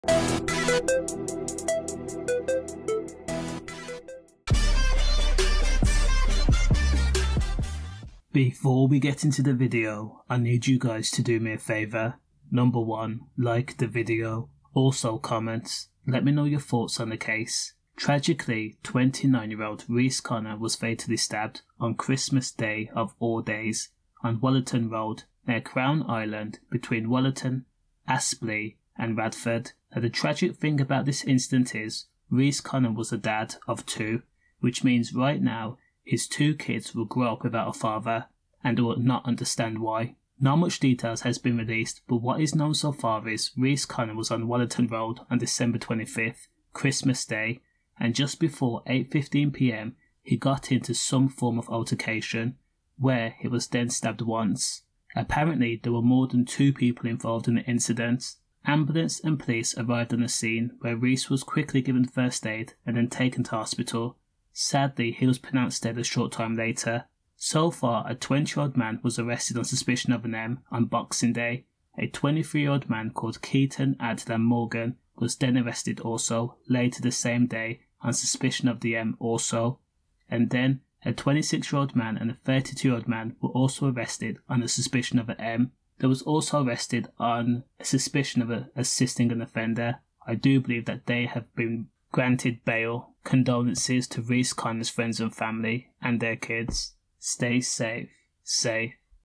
dad, Reece Connor fatally st**bed on Christmas Day
the family of stab victim Reece Connor have paid tribute to him in a statement calling him an "amazing father". The 29-year-old, whose picture has been released by his relatives for the first time, died on Christmas Day after an incident in Wollaton Road, near the Crown Island, between Wollaton, Aspley and Radford.

Described as a "strong-willed man who worked so hard to get the things he wanted in life", Mr Connor was a father to two sons. He was taken to hospital via ambulance after the incident but died of his injuries.

The full statement from his family said: “Reece was an amazing father of two beautiful boys who he cherished with his whole life. He was the funniest, kind and caring man who would do anything for the people he loved. He was the type of man who would drop e